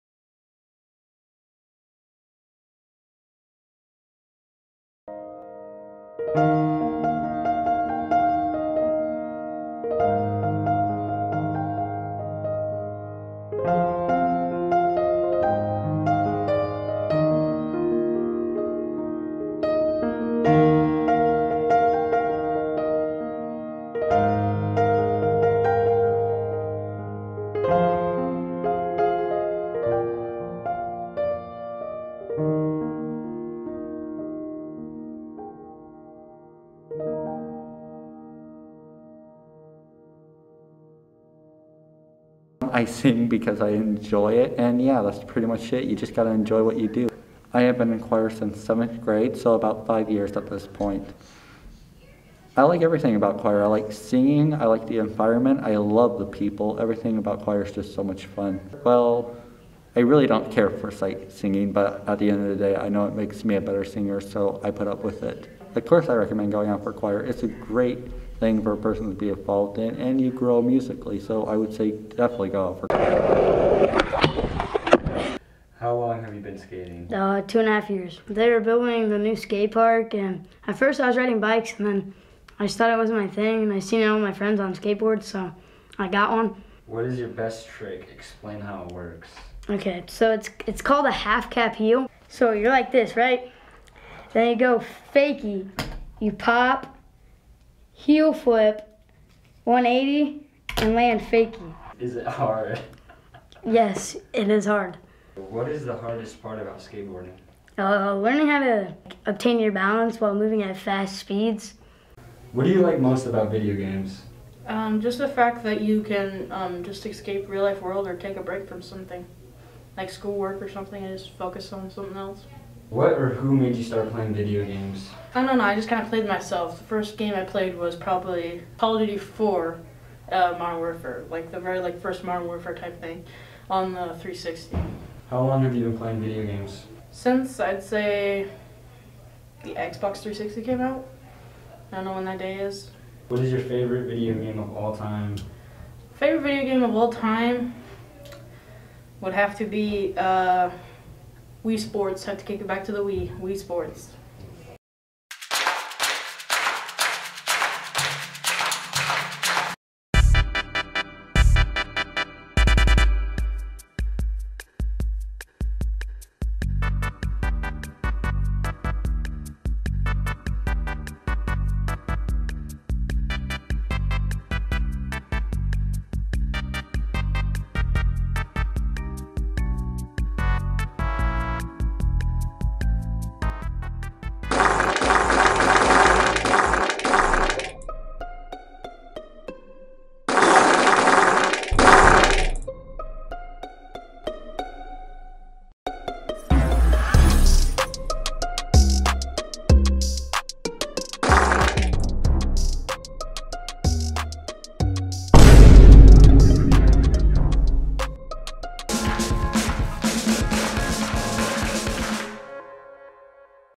Bulldog Bites #46, May 12, 2021
5 shot sequence/interviews and a silly chase scene. Featuring work by Kyle Hubbell, Ben Duque, Trey Huizenga and Kaden Rolling and interviews of Kyle, Taye Hassman, and Clayton Domsch.